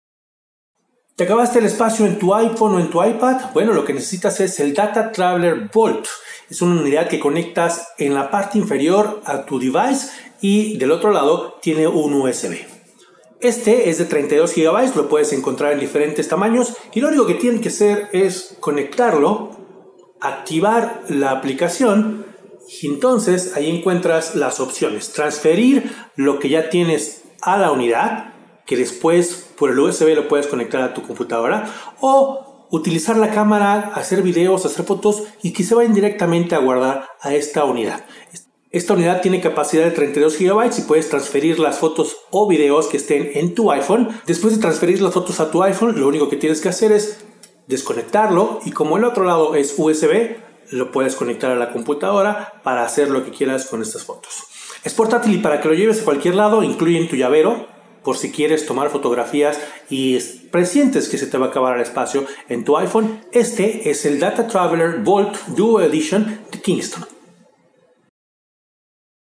Kingston DataTraveler Bolt Duo
¿Se está acabando el espacio de tu iPhone o iPad? Con el DataTraveler Bolt Duo de Kingston puedes solucionar el problema, aumentanto espacio o transfiriendo las fotos y videos de manera sencilla.